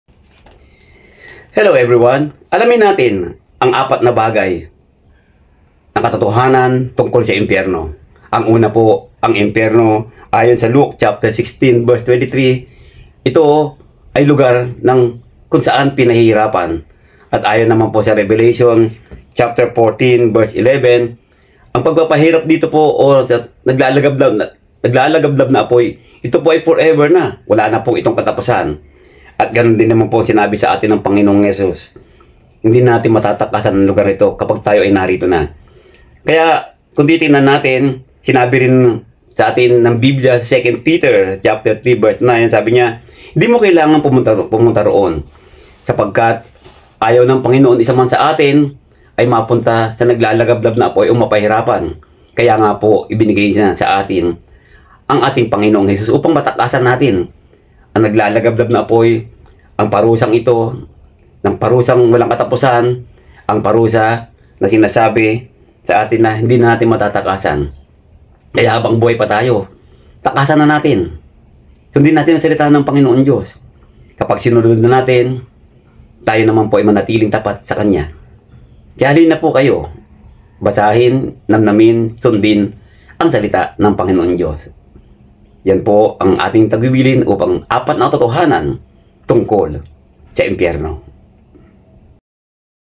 4 na Katotohanan na Hindi natin Matatakasan tungkol sa Impiyerno
Alamin ang Apat na katotohanan na nakasulat sa Bibliya tungkol sa Impiyerno na hindi natin matatakasan.